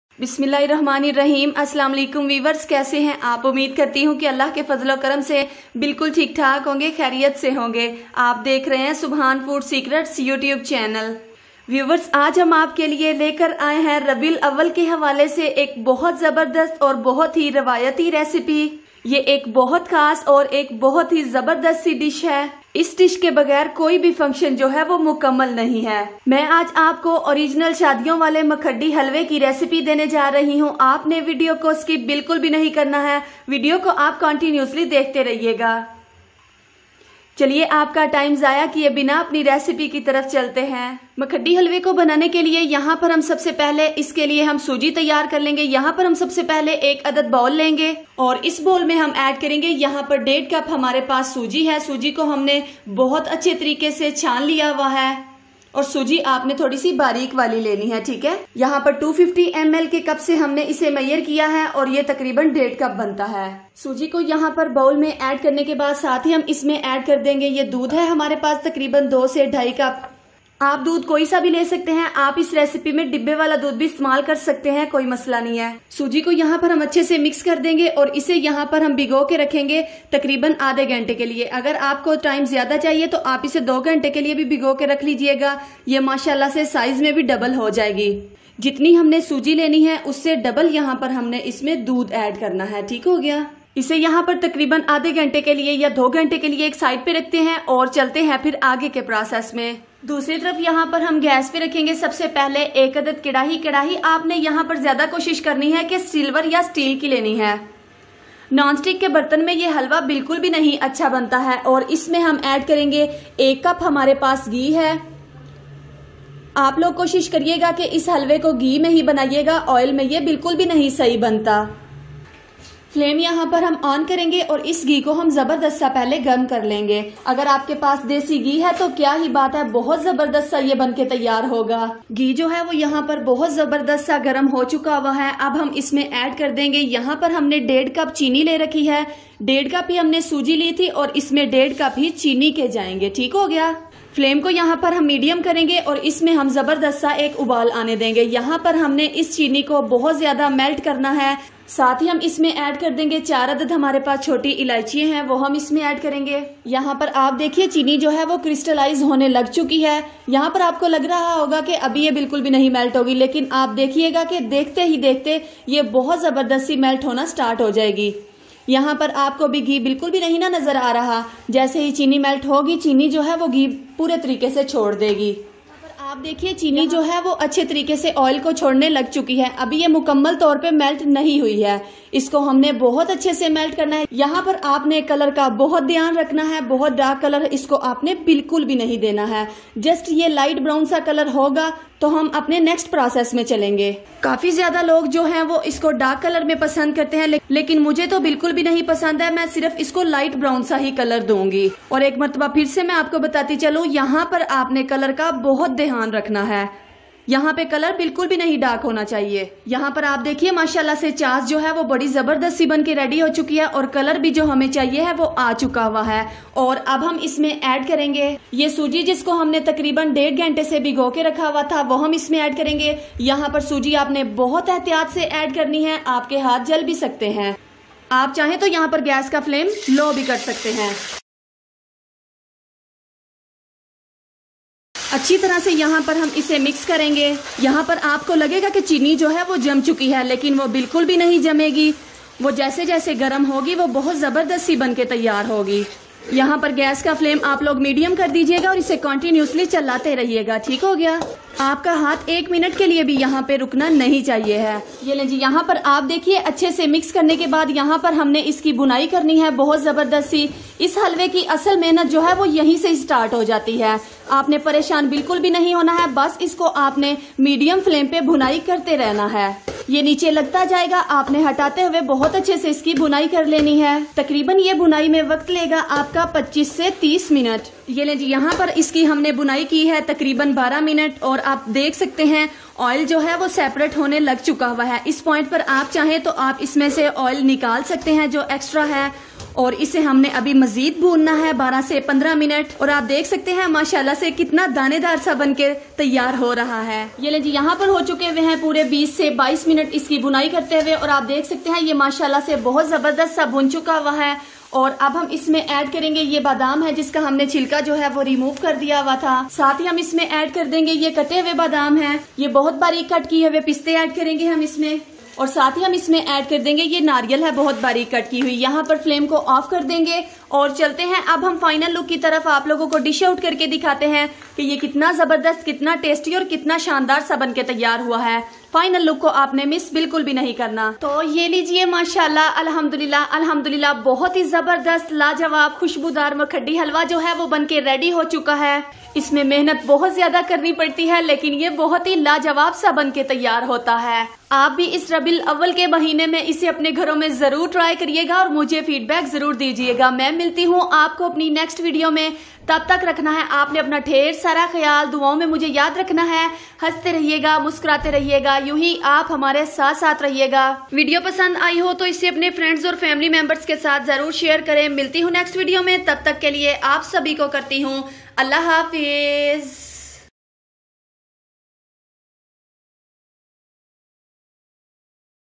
Makhandi Halwa Recipe|12 Rabi-Ul-Awal Special Recipe|Suji Halwa Recipe|By Subhan Food
Hi dosto aj ki is video main hum ap k liye lty kr ay hain special makhndii halwa ki recipe isy bnaya bhut zyada asan ha or bhut jildi bn k tyar porta ha ap b isy 12 ki khaas recipes main bnaiye ga ye bhut zbrdast dish ha agr video achi lgy to plz isy like kriye ga agr ap mery channel py new hain subscribe kr kr dijiye ga or mujy feed back zroor diiiye ga jazakAllah

10 Minute Chicken Recipe || Quick & Easy Chicken Recipe By Subhan Food Secrets

MakhandiHalwaRecipe HalwaRecipe 12Rabi-Ul-AwalSpecialRecipe#